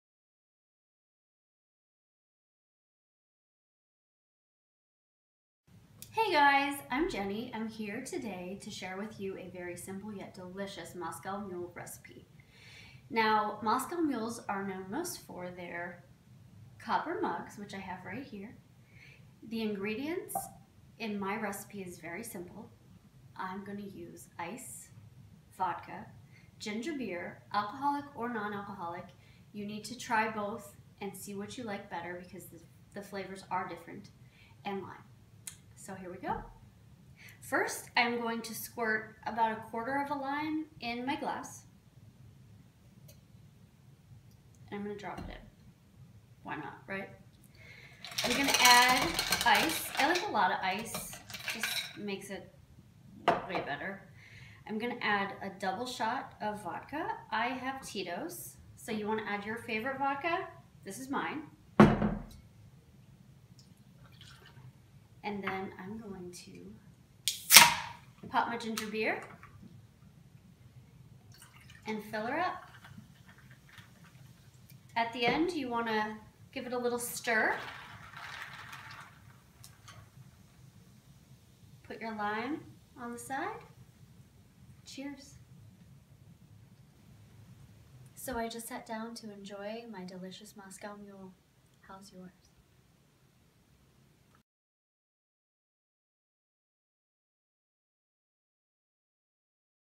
How to Make a Simple Moscow Mule
How to make a easy Moscow Mule with Vodka, Ginger Beer, Lime and a Copper Mug Simple andEasy